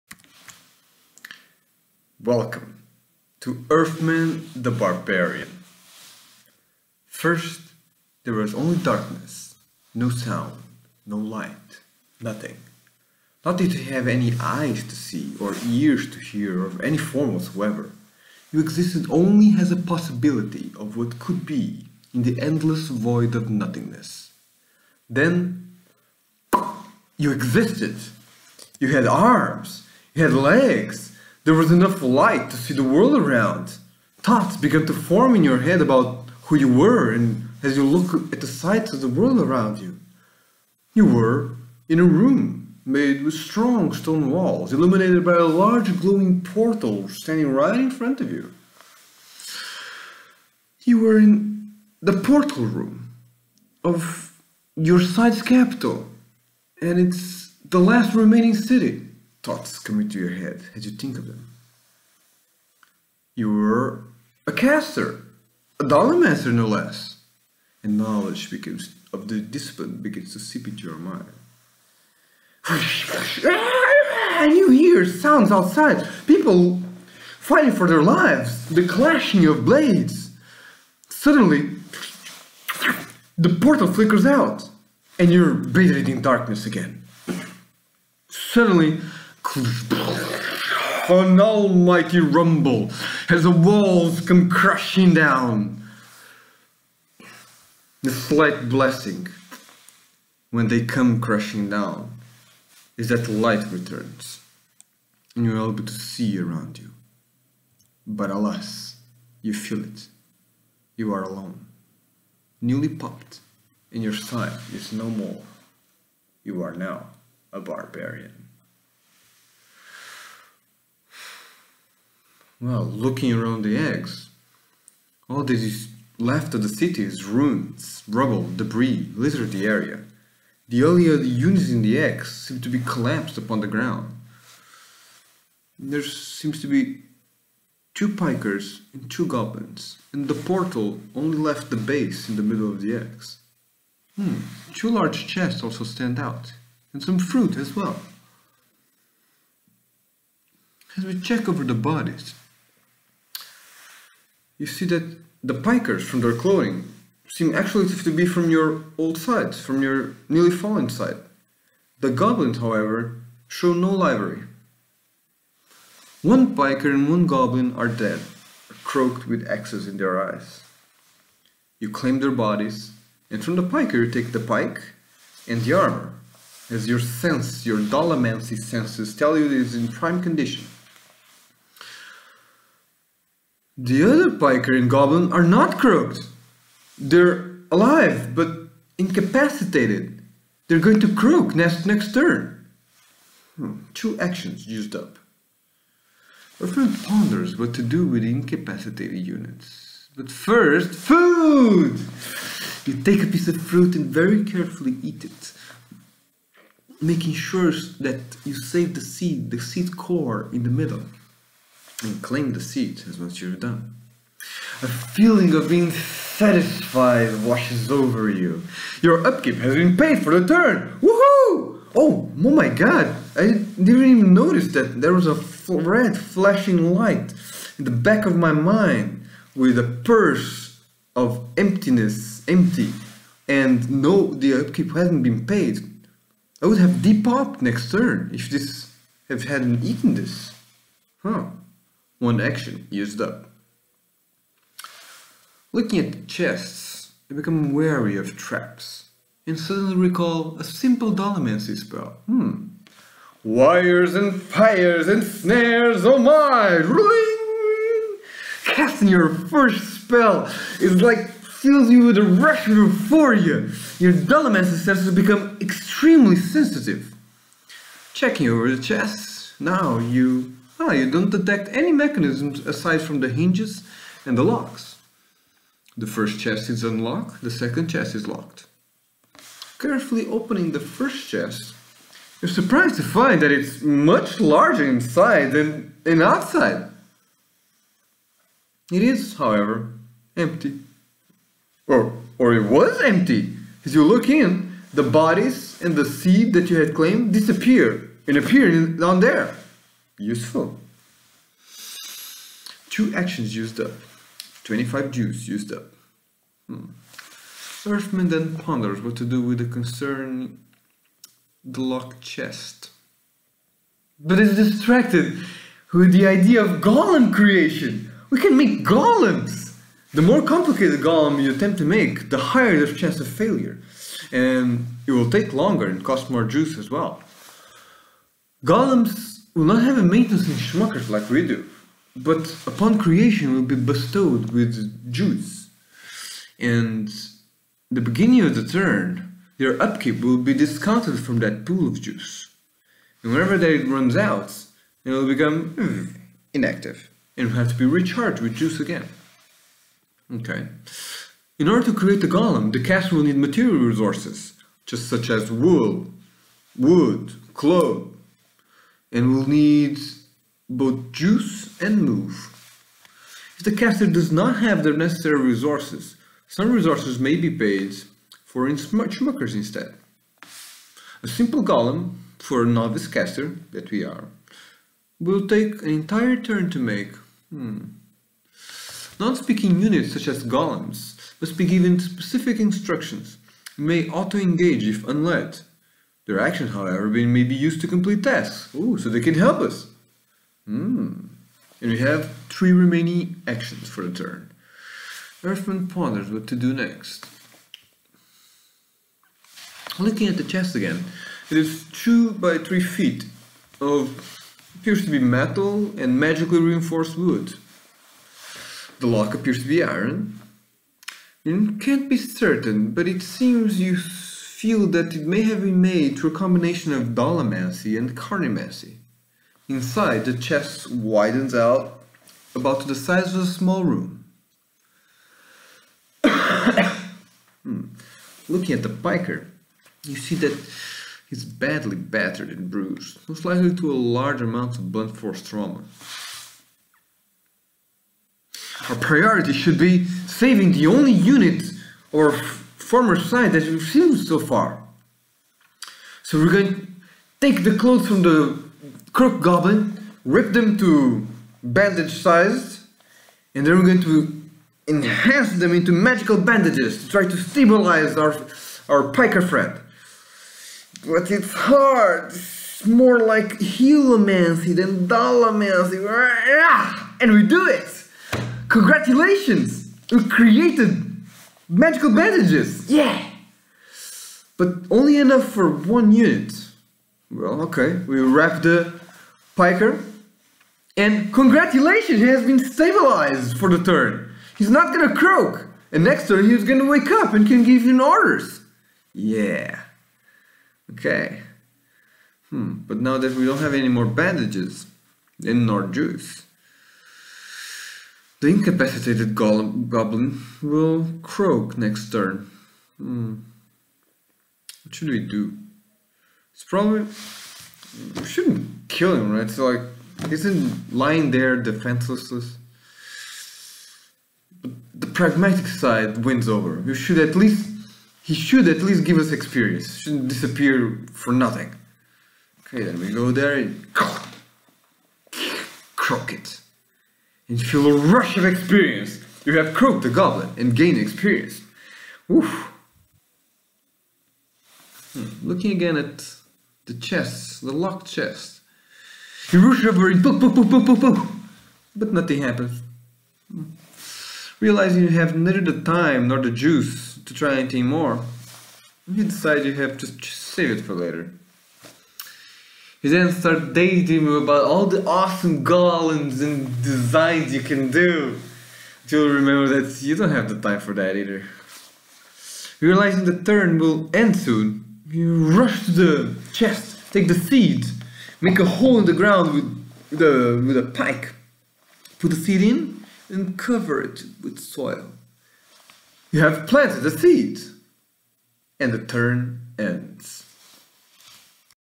Erfman the Barbarian - Turn 1
This a narration of the forum game Erfman the Barbarian. a game where anyone can chime in, and suggest actions for Erfman to do, and then if there is consensus in Erfman's head, he goes and does it :))

If you were entertained, tell me by liking and commenting and I'll keep on going!